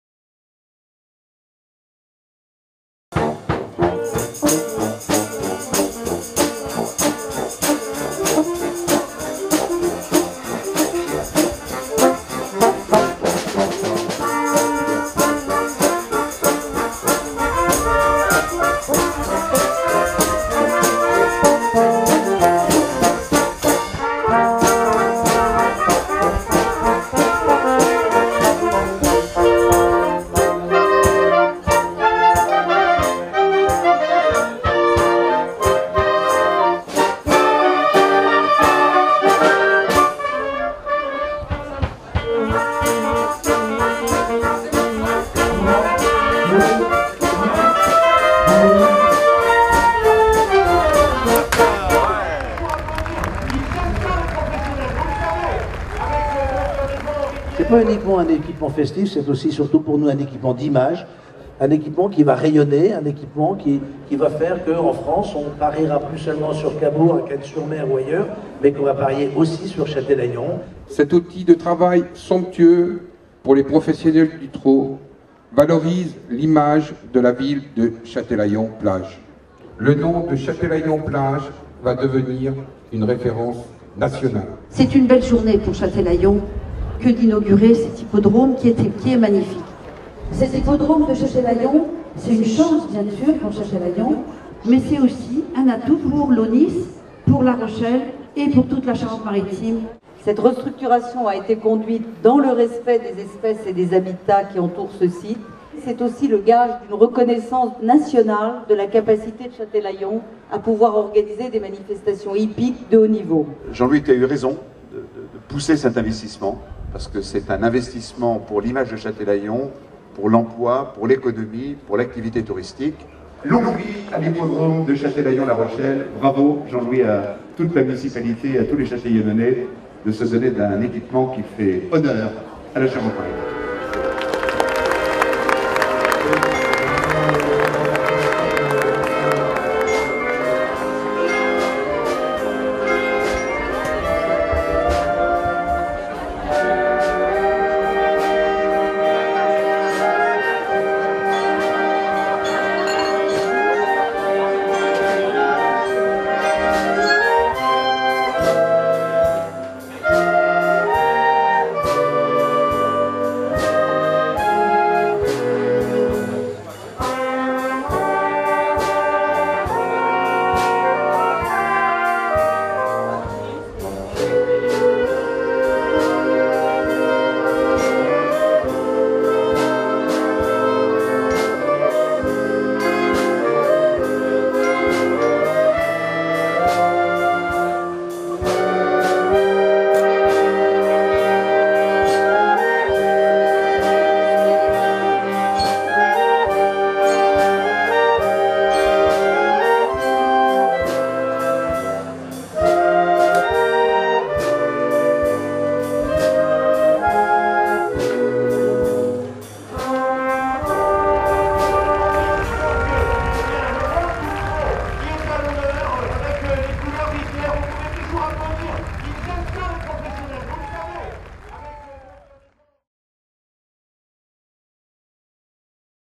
Inauguration de l'Hippodrome de Châtelaillon-Plage
Le Nouvel hippodrome de Châtelaillon-Plage a été inauguré Samedi 29 juin 2013. Retrouvez cet évènement en image.